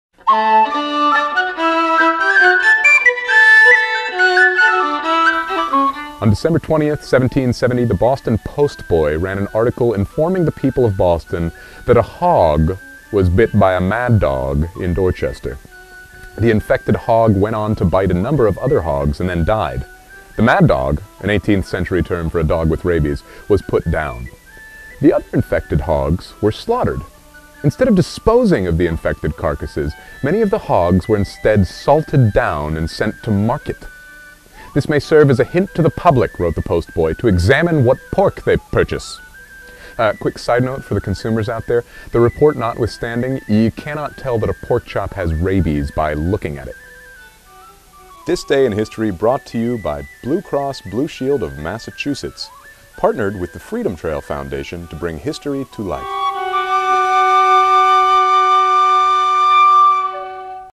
This Day In History - December 20 - Freedom Trail Foundation
Freedom Trail Player Daniel Berger-Jones, who portrays Dr. Samuel Prescott, recounts a rather odd rabies outbreak for This Day In History.